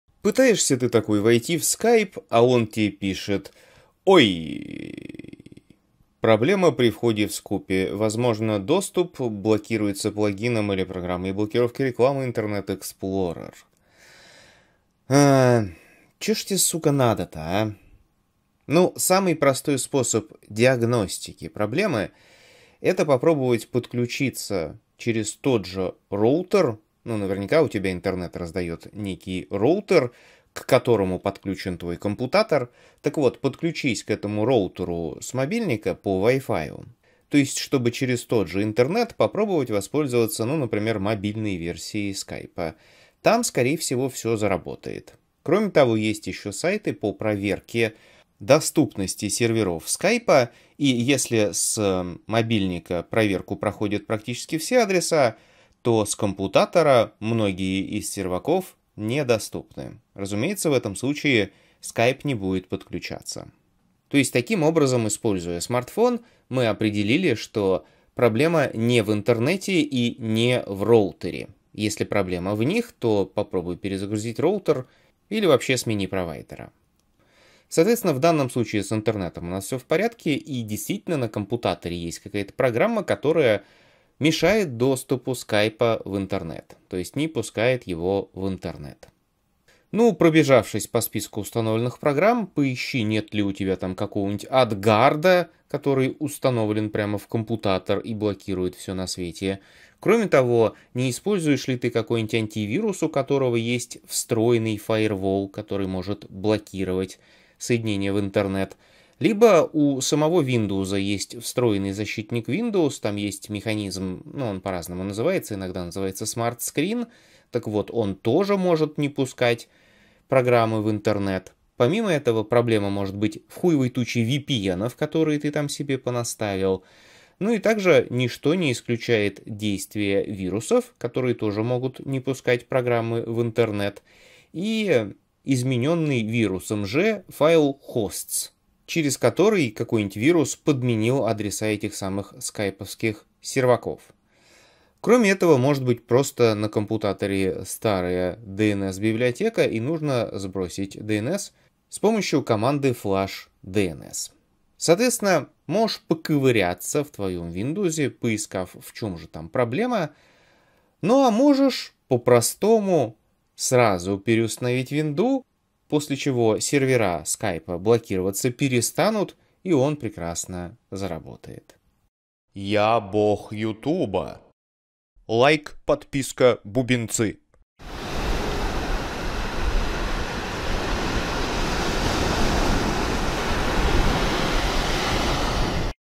🚩 Проблема входа в Skype
Сайт для проверки Проблема при входе в Skype. Возможно, доступ блокируется плагином или программой блокировки рекламы Internet Explorer. Проверьте настройки Internet Explorer и повторите попытку позже.